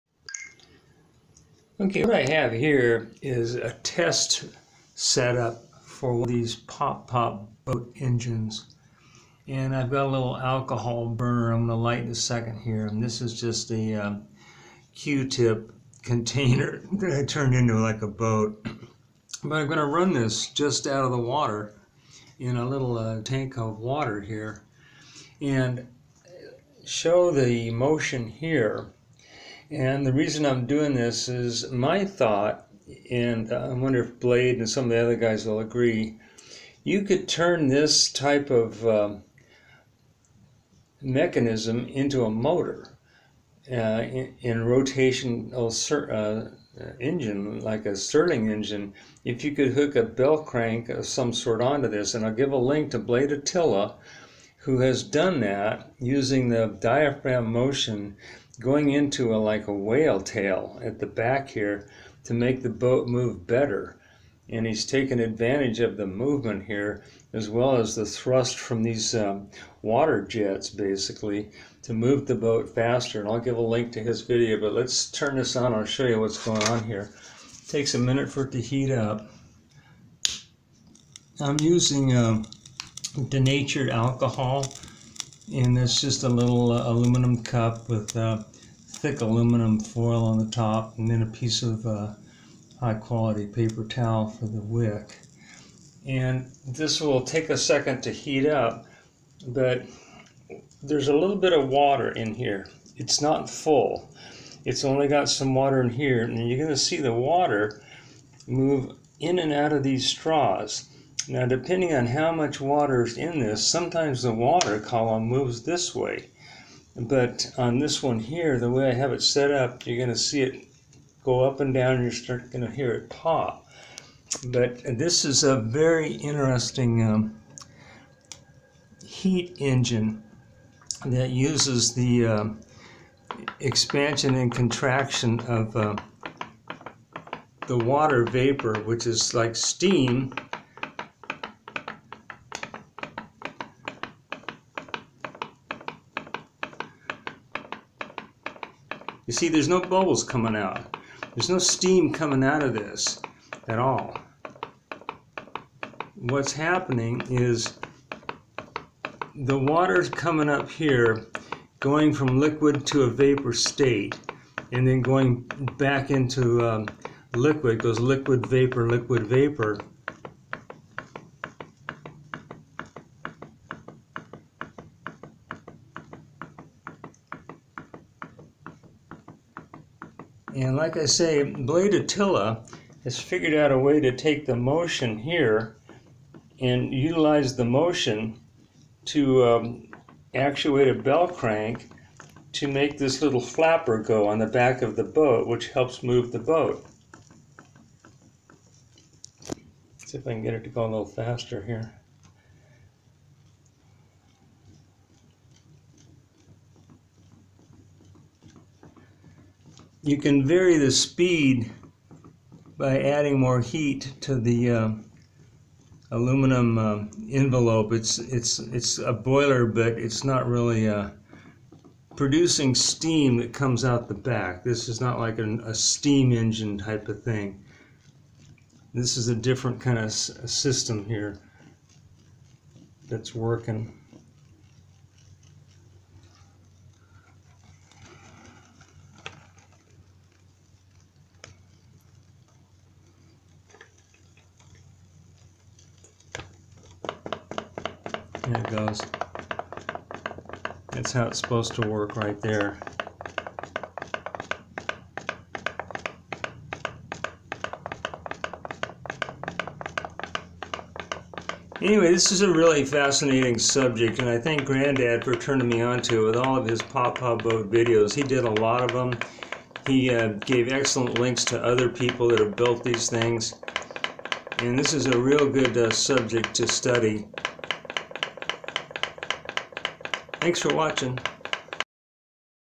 Pop Pop Boat Engine -- Dry dock testing
This shows a common 'Pop Pop Boat' engine being tested out of the water.  It shows how the water in the engine moves up and down both exhaust straws.
This is the Blade Attila video where he uses the motion of the engine diaphram to move a 'whale tail' at the back of his boat